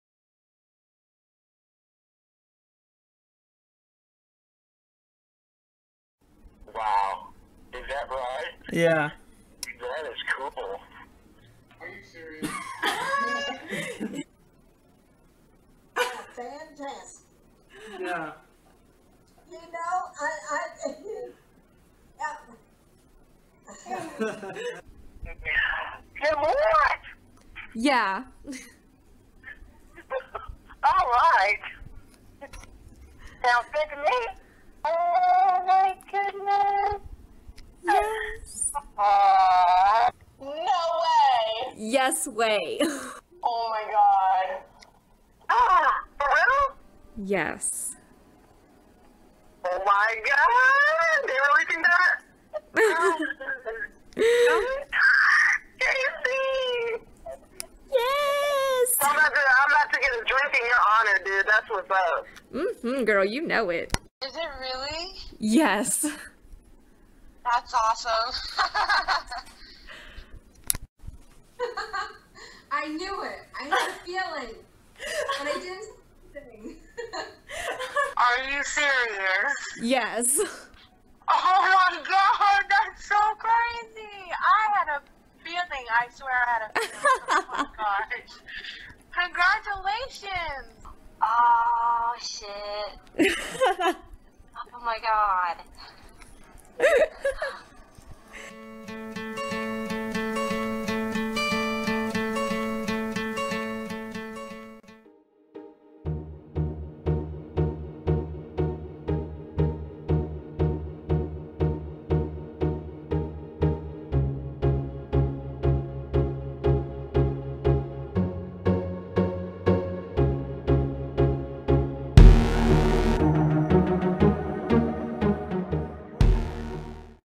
Surprise twin pregnancy announcement! | A MUST SEE!
This is our official pregnancy announcement.

Hey, hey, hey, Youtube views and vloggers! Thank you for checking us out. We used to be Hudson's plus one but we recently found out that we are expectin